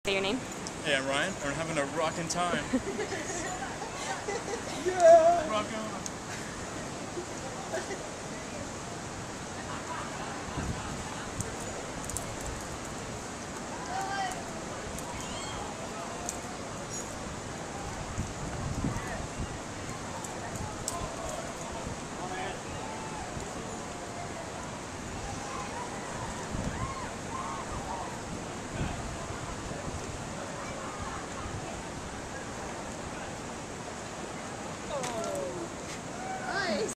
Ryan Genest climbing at the Whitewater Center
Check out my climb at the Whitewater Center this past Saturday!